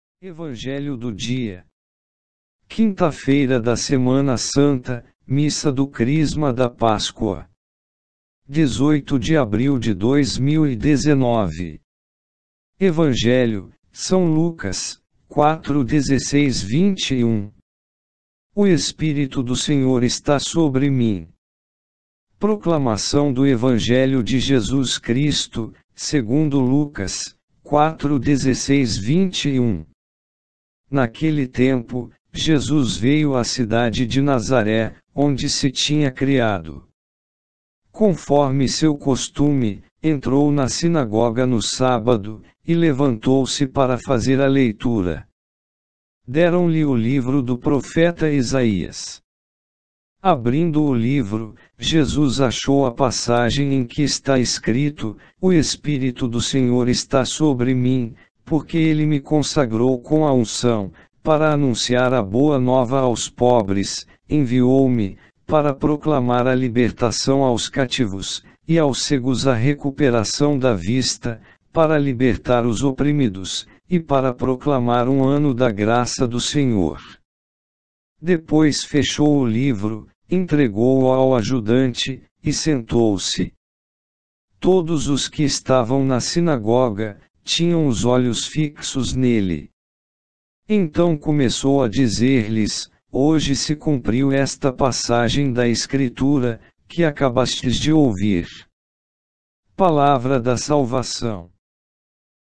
EVANGELHO DO DIA, 18 DE ABRIL DE 2019
Quinta-feira da Semana Santa - Missa do Crisma da Páscoa.

18 de Abril de 2019.

Evangelho - São Lucas, 4,16-21.

O Espírito do Senhor está sobre mim.

 Proclamação do Evangelho de Jesus Cristo, segundo Lucas, 4,16-21.

Naquele tempo:
16Jesus veio à cidade de Nazaré, onde se tinha criado.
Conforme seu costume, entrou na sinagoga no sábado,
e levantou-se para fazer a leitura.

17Deram-lhe o livro do profeta Isaías.
Abrindo o livro,
Jesus achou a passagem em que está escrito:

18'O Espírito do Senhor está sobre mim,
porque ele me consagrou com a unção
para anunciar a Boa Nova aos pobres;
enviou-me para proclamar a libertação aos cativos
e aos cegos a recuperação da vista;
para libertar os oprimidos,

19e para proclamar um ano da graça do Senhor.

20Depois fechou o livro,
entregou-o ao ajudante, e sentou-se.
Todos os que estavam na sinagoga
tinham os olhos fixos nele.

21Então começou a dizer-lhes:
'Hoje se cumpriu esta passagem da Escritura
que acabastes de ouvir.